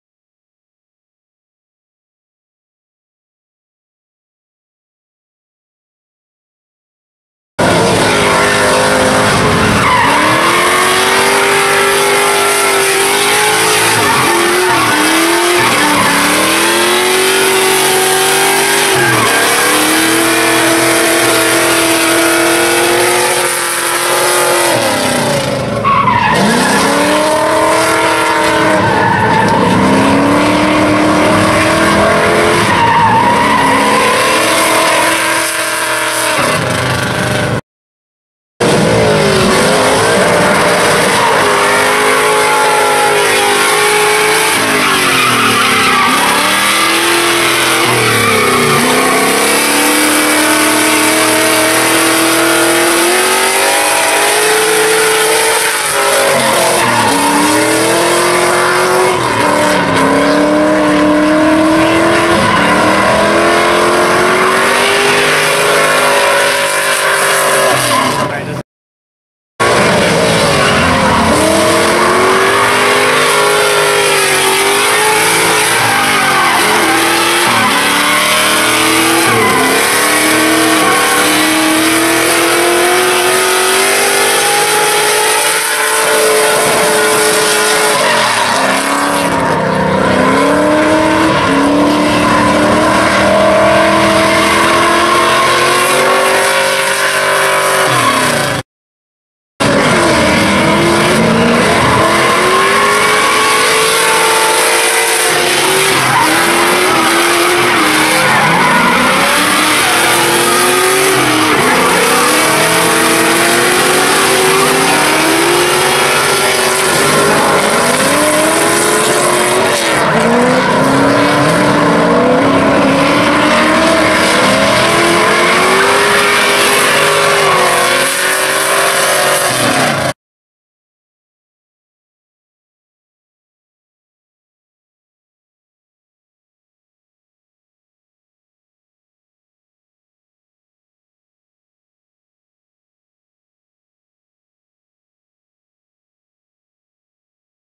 Barry Clapp - 4 runs
September 24, 2016, Turismo Drift Round 3, Tennessee Smokies Stadium, Kodiak Tennessee